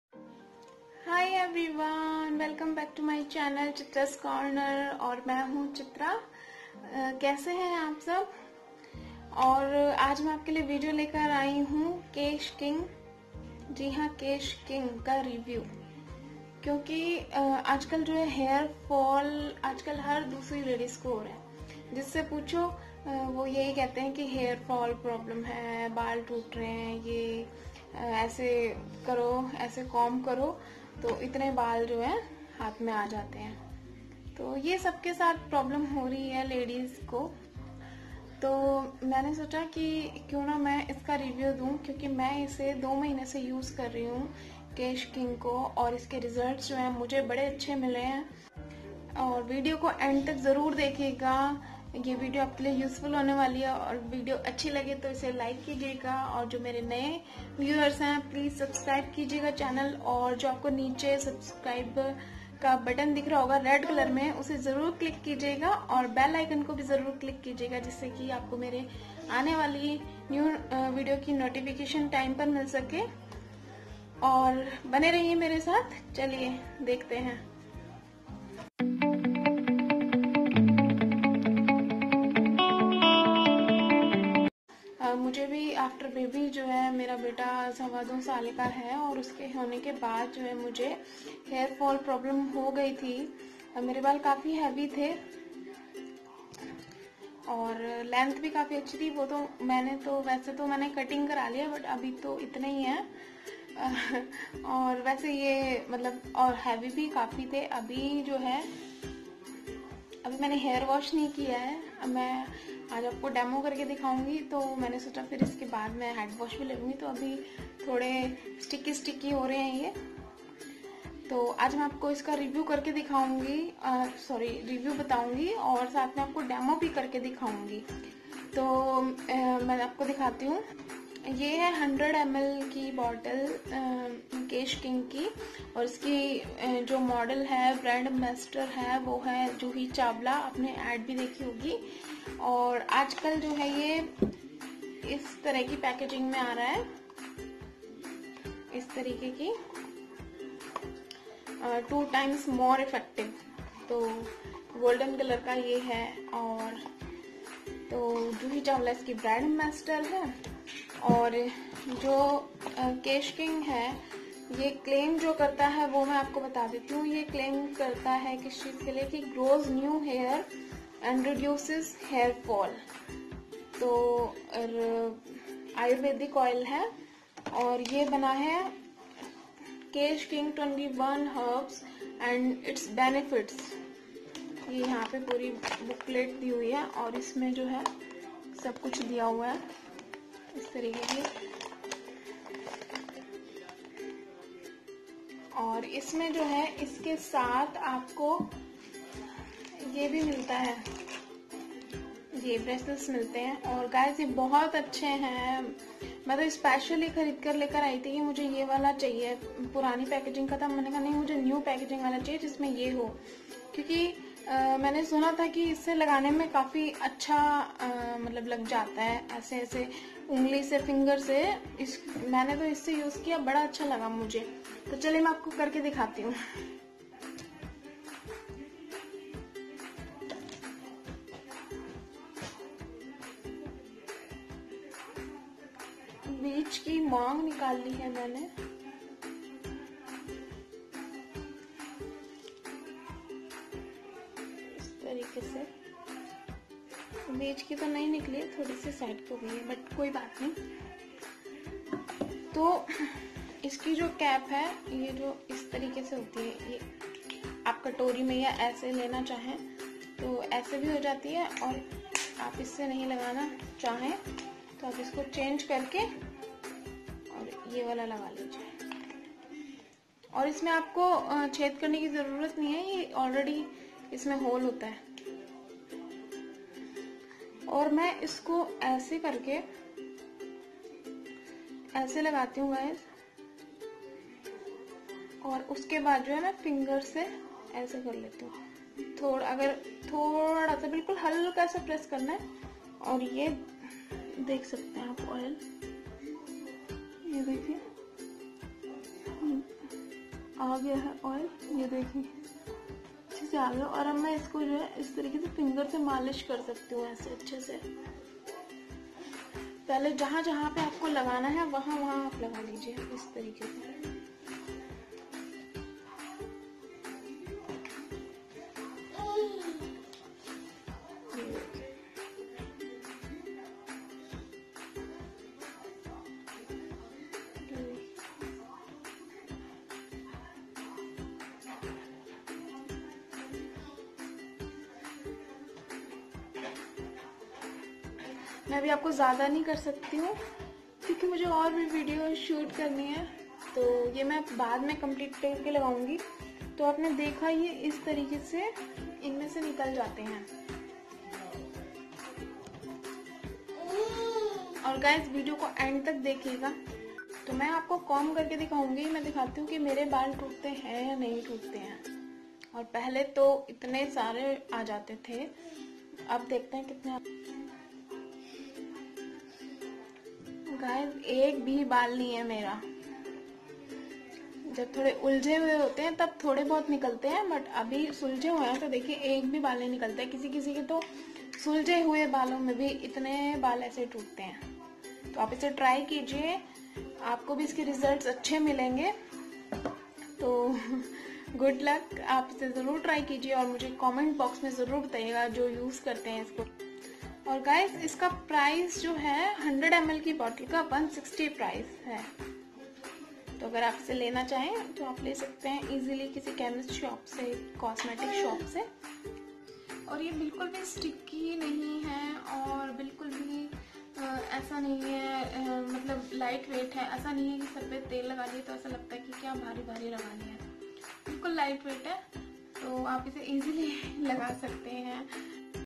Kesh King Ayurvedic Hair Oil Full Review with Demo | Review of Kesh King Hair Oil | 2019
Hi Everyone,

Thank u 😄😘

Bundles of love